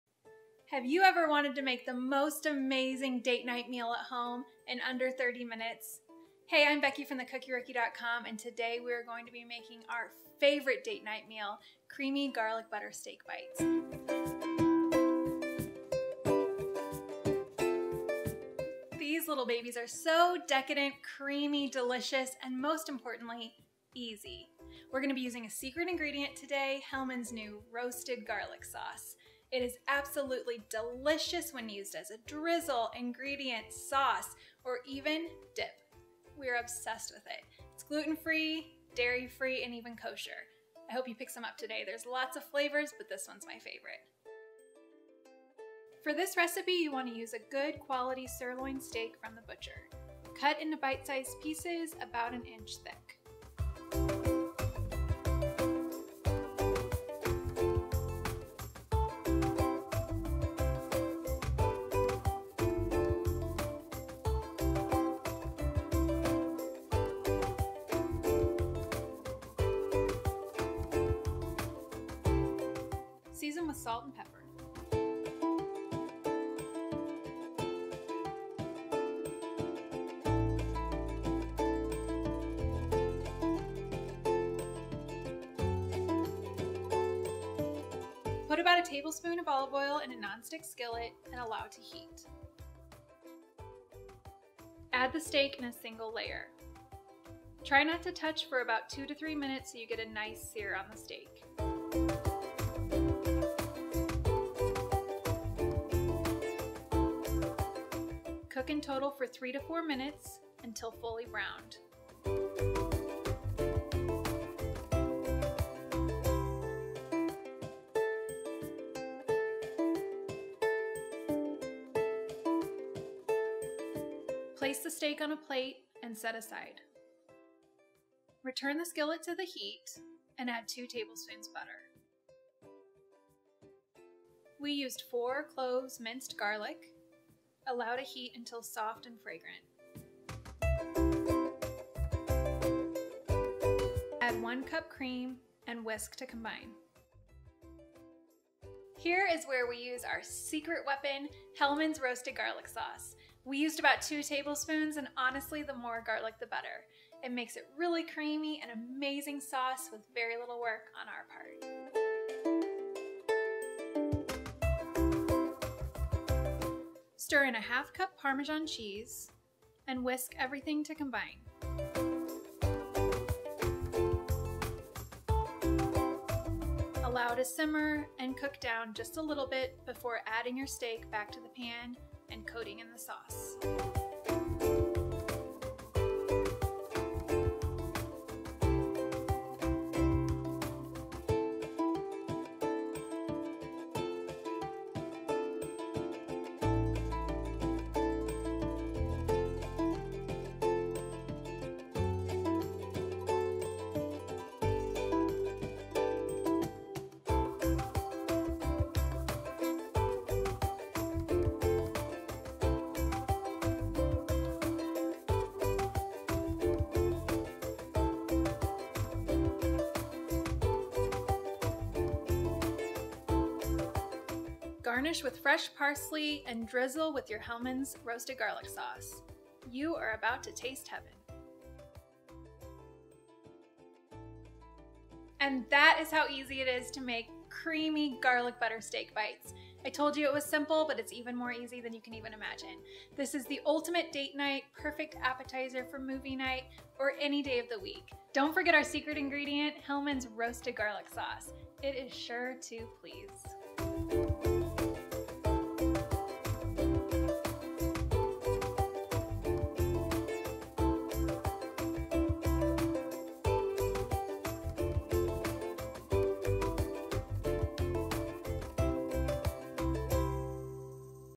Garlic Butter Steak Bites
We are obsessed with the Hellmann's new drizzle sauces! We used Hellmann's Roasted Garlic Sauce to make our famous Garlic Butter Steak Bites. SO creamy and delicious.

RECIPE:
2 lbs. good quality sirloin steak
salt and pepper
1 tablespoon olive oil
2 tablespoons butter
4 cloves minced garlic
1 cup heavy cream
1 tablespoon Hellmann’s Roasted Garlic Sauce (plus more for serving)
1/2 cup grated parmesan cheese
Freshly chopped parsley

Cut sirloin into 1 inch cubes.
Heat olive oil in a large saucepan over medium heat.
Add steak to skillet and cook in a single layer. All to cook undisturbed for 2-3 minutes until seared.
Use a wooden spoon to turn the steak and cook for 1-2 minutes more until the steak bites are cooked through to your liking and fully browned.
Remove the steak from the heat and set aside on a foil lined plate. Do not drain the skillet.
Add butter to the same skillet and allow to melt.
Add the garlic and cook until fragrant, 2-3 minutes.
Add the cream and whisk to combine.
Stir in the Hellmann’s Roasted Garlic Sauce.
Stir in the parmesan cheese and allow to melt. Whisk to combine. Allow to simmer and thicken for 2-3 minutes.
Add the steak back to the skillet and toss in the sauce. Cook just long enough to heat through.
Sprinkle with chopped parsley.
Drizzle with Hellmann’s Sauce and serve with some for dipping. Enjoy!